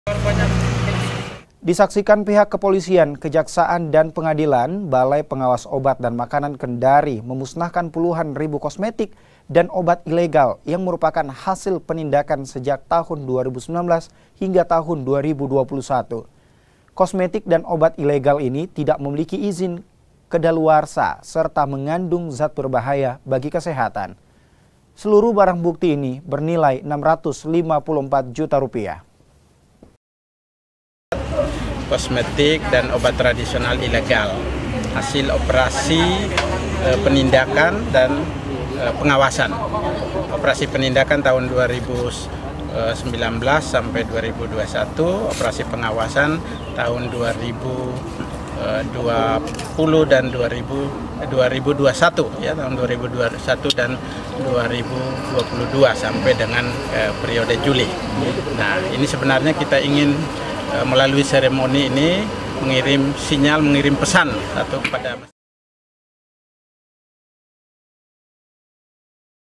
BPOM Kendari Musnahkan Ribuan Kosmetik Dan Obat Ilegal
KENDARI, KOMPAS.TV - Balai pengawas obat dan makanan kendari memusnahkan ribuan paket kosmetik dan obat-obatan ilegal yang berbahaya bagi kesehatan.

Disaksikan pihak kepolisian kejaksaan dan pengadilan balai pengawas obat dan makanan kendari memusnahkan puluhan ribu kosmetik dan obat ilegal yang merupakan hasil penindakan sejak tahun 2019 hingga tahun 2021.

Kosmetik dan obat ilegal ini tidak memiliki izin kedaluarsa serta mengandung zat berbahaya bagi kesehatan. Seluruh barang bukti ini bernilai 654 juta rupiah.